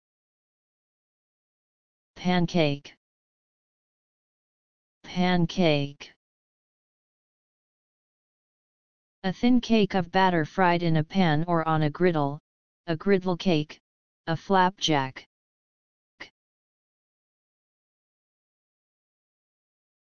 Pancake Meaning
A thin cake of batter fried in a pan or on a griddle; a griddlecake; a flapjack.

In this video, you will learn "Pancake" Meaning and pronunciation in English.

Wordy Affair